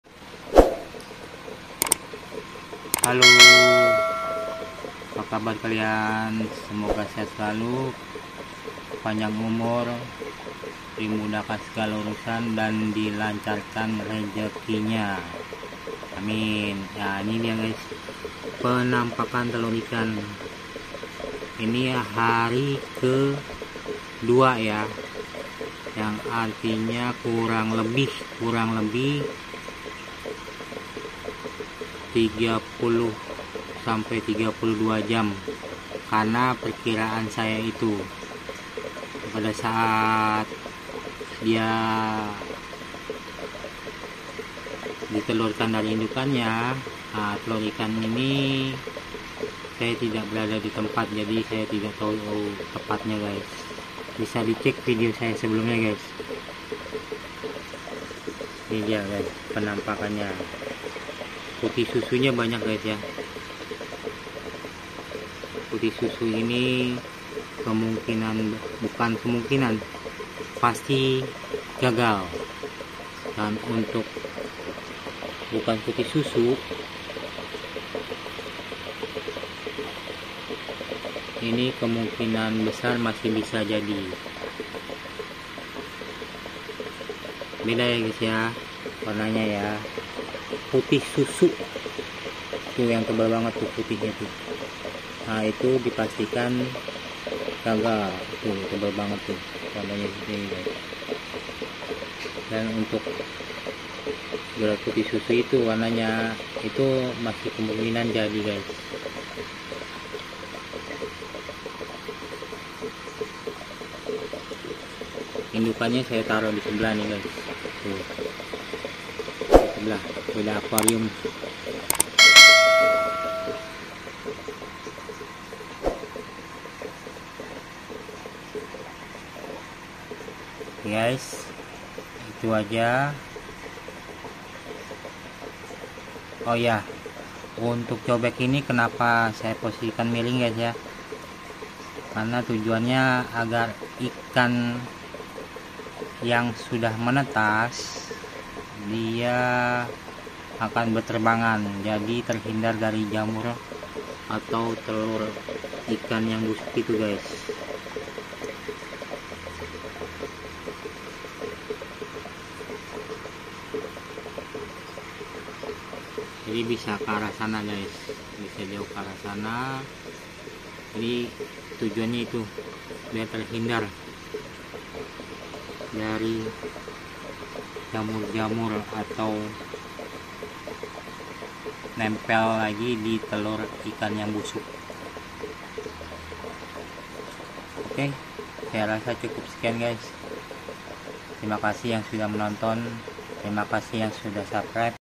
PROSES PENETASAN TELUR IKAN LOUHAN CENCU INDONESIA x KAMFA F2 ORI THAILAND || BREEDING LOUHAN
Hari ke 2 Bertelur
Breeding Louhan Ke 2 dari pasangan
Cencu Indonesia Betina x KAMFA F2 Ori Thailand

Utk Breeding Louhan ke 1 dari pasangan yg sama
silahkan tonton di channel sy @marchelRio

TERIMA KASIH
SEMOGA BERMANFAAT 🙏